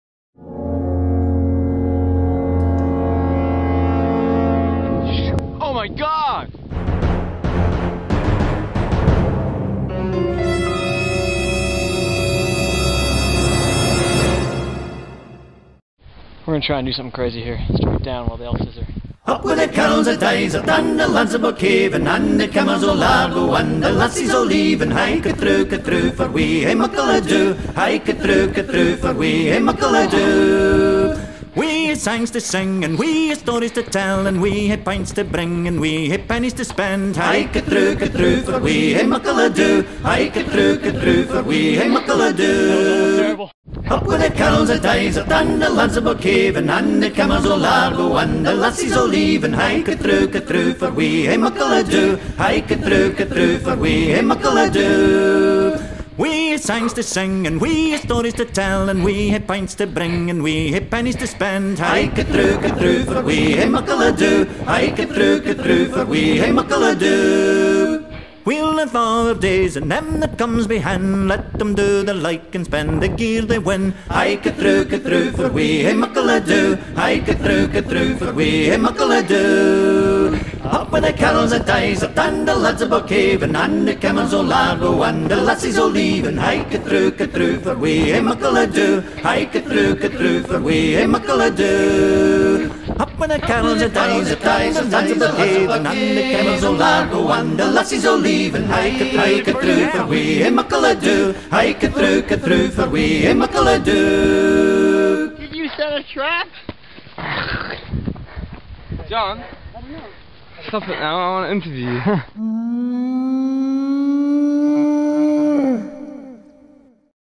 2009-05-30 Hiking and Hill Running.mov
Hiking and Running the hills north of Hohhot, Nei Meng China, Inner Mongolia.